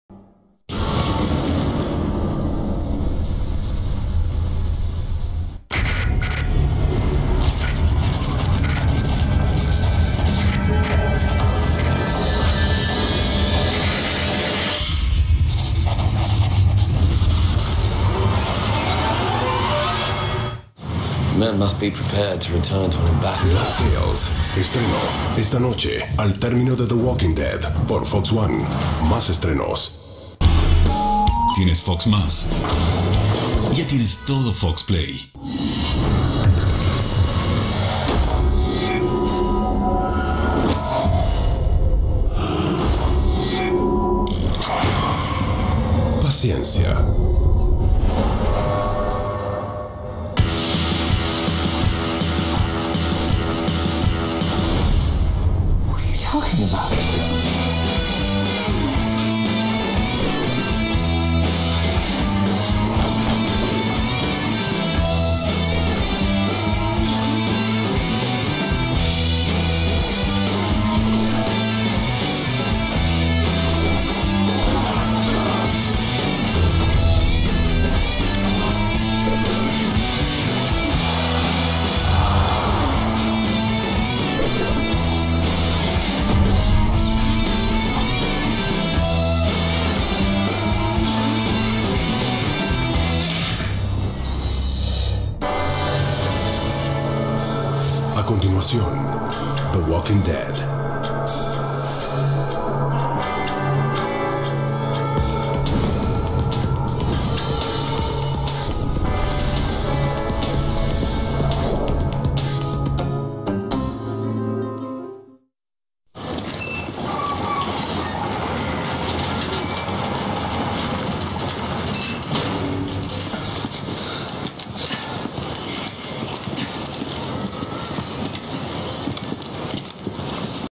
FOX1 - Pacote gráfico (2014-2017)
¡Demorou mas está aqui! Compilação do pacote gráfico do canal FOX1, de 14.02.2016. Feed México.
Desde 2017 o canal se chama FOX Premium Series (LatAm), FOX Premium 1 (Brasil).